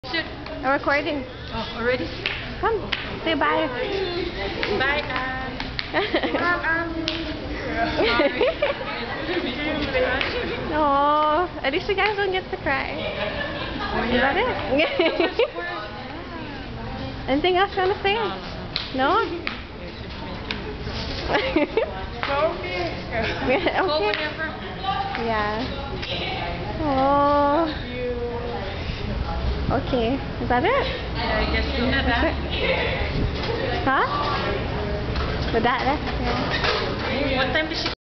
Babidoll's Farewell #4
Third to arrive late to see Ann off are Lena, Pav, and Rosalina.